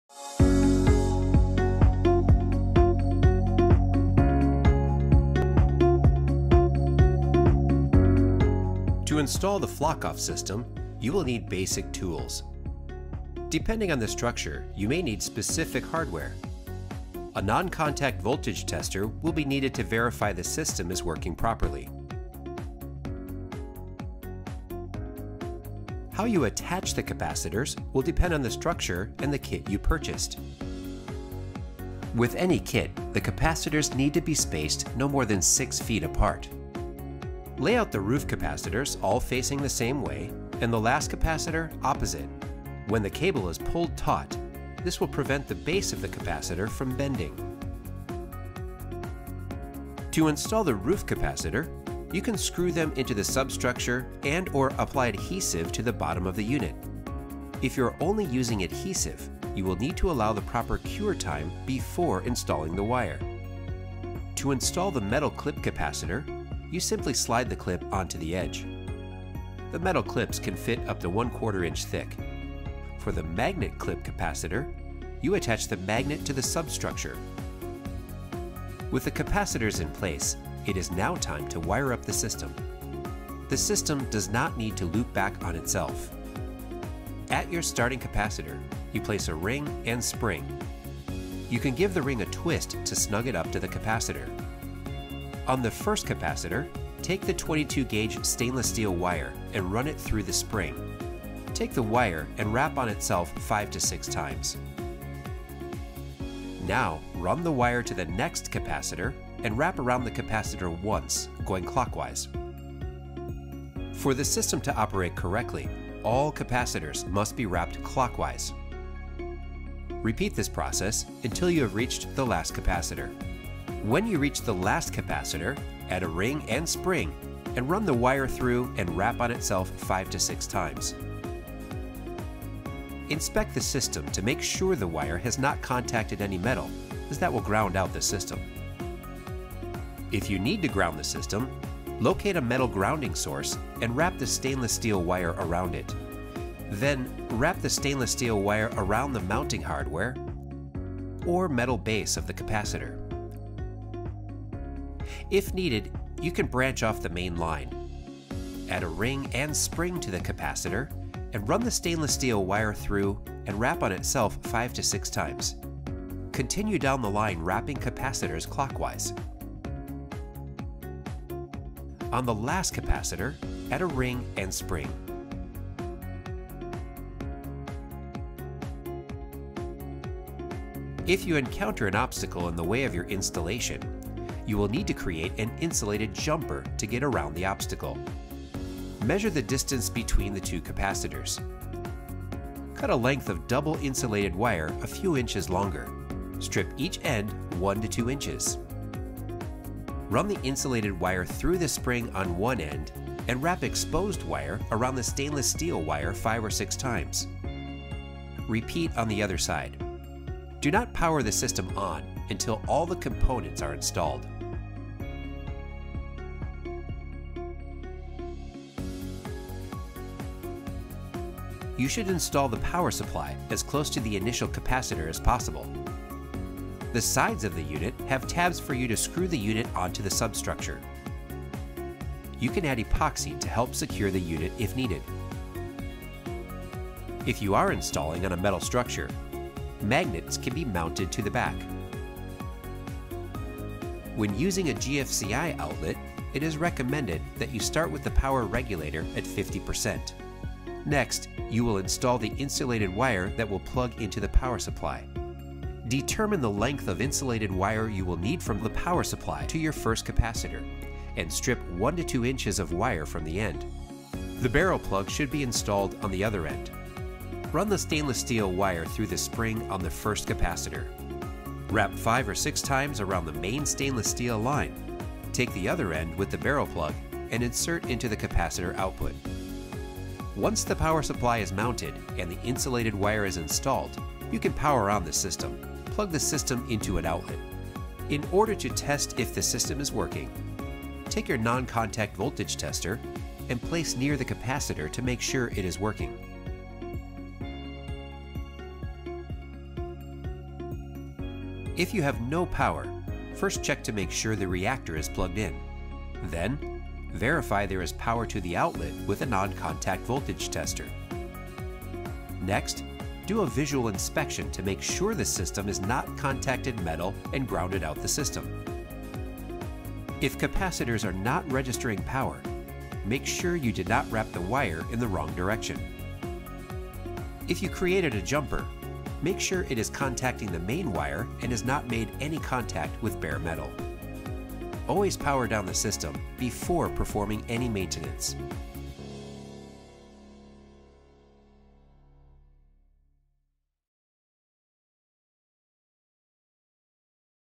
Symterra - Commercial Installation
When birds fly near, Symterra (formerly known as Flock Off) disrupts a key protein in their bodies used to navigate. It affects mineral crystals present their beaks and wave receptors in their ears, eyes, and brain.  This momentarily halts their ability to judge speed, direction, and distance. The birds experience what humans might feel as “vertigo” within 2 meters of the system…AND simply cannot land. Check out how the system is installed.